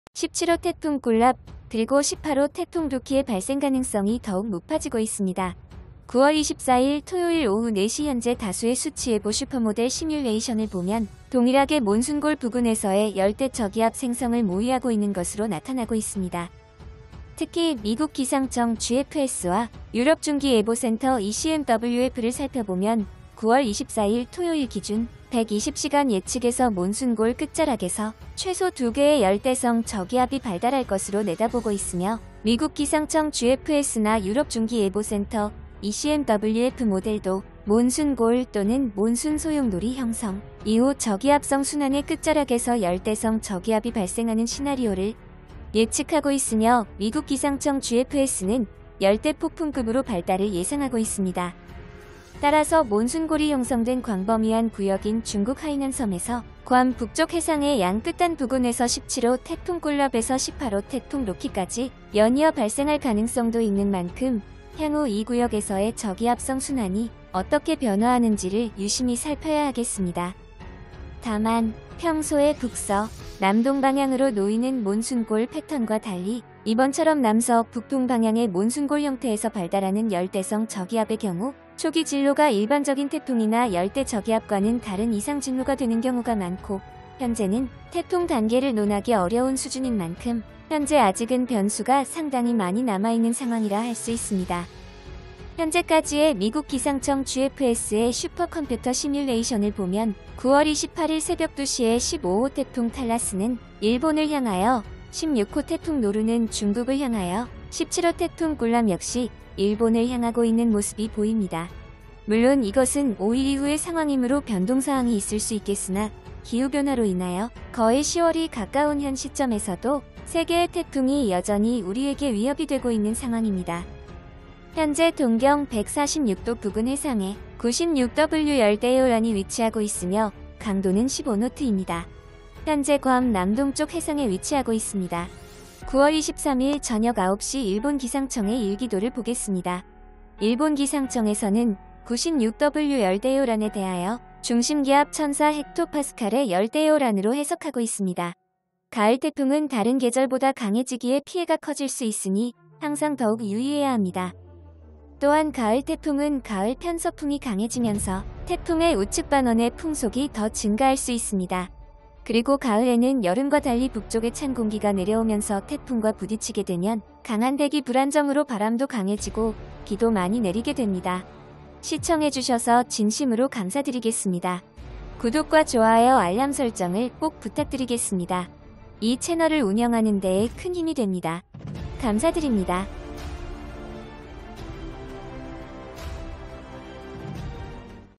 15호 탈라스 17호 꿀랍 18호 로키 ▶일본 15-16-17-18호 태풍 발생 러쉬
15호 탈라스 17호 꿀랍 18호 로키 ▶일본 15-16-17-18호  태풍 발생 러쉬
17호 태풍 꿀랍 그리고 18호 태풍 로키의 발생가능성이 더욱 높아지고 있습니다.9월24일 토요일 오후 4시 현재 다수의 수치예보 슈퍼모델 시뮬레이션을 보면 동일하게 몬순골 부근에서의 열대저기압 생성을 모의하고 있는 것으로 나타나고 있습니다